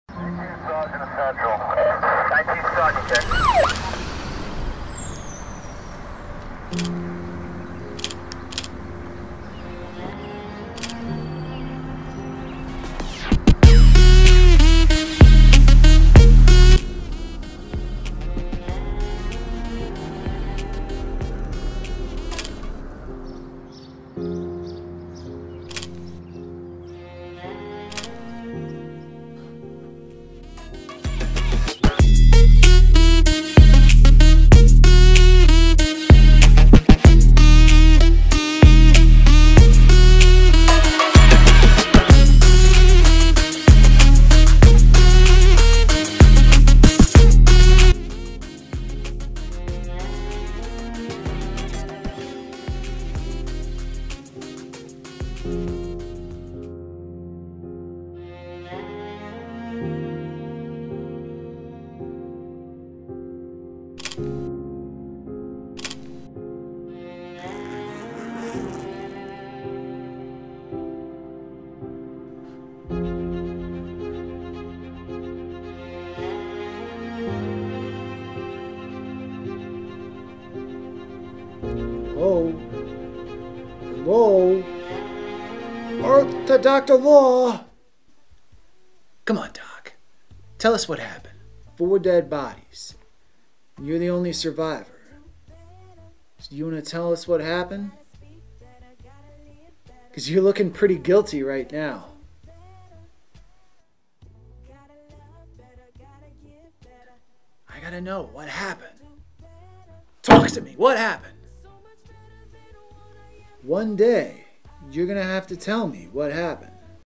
Jeckyl A Work In Progress LOL
Short little progress on a goofy little project. The goal is to give myself one hour to come up with a short story and script... many hours to film but has to fit in to my actual work schedule. This isn't done yet. There's another scene to throw in. But I need to upload something so I can put it to rest.